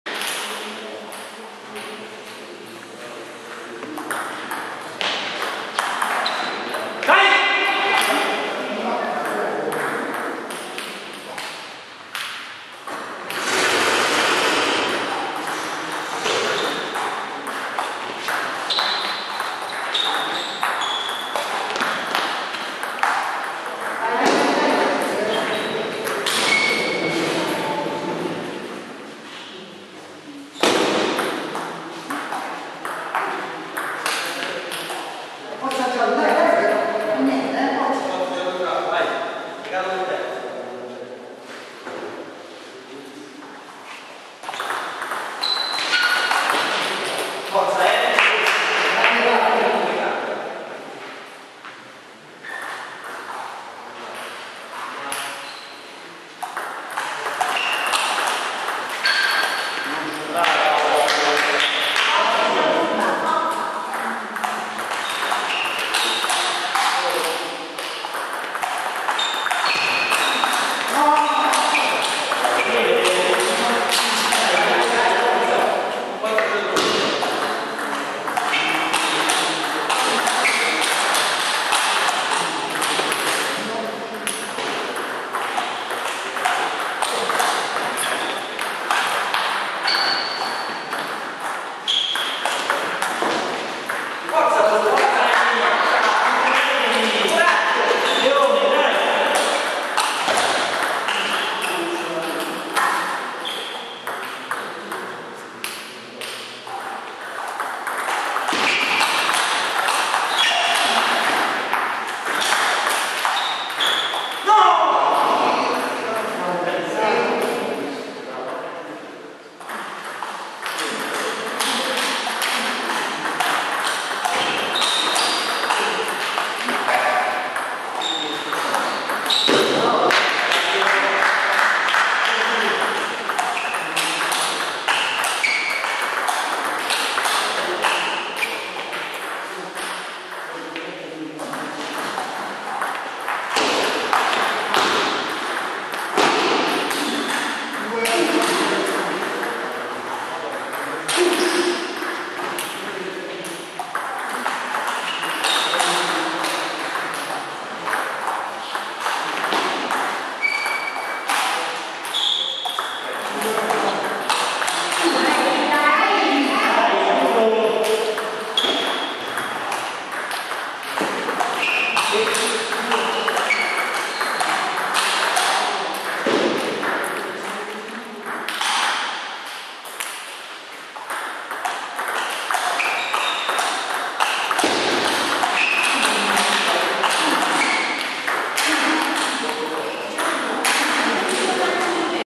ANGIOLELLA vs FRATTALI - 2° SET
Tennistavolo. Stagione 2011/2012.
Campionato di C2 Regione Lazio.
22 maggio 2011
Andata Playoff per la promozione in C1.
Incontro tra Castello Giallo e Hernicus Anagni.
Monia Franchi (Castello) vs Gianluca Frattali (Anagni). Secondo set.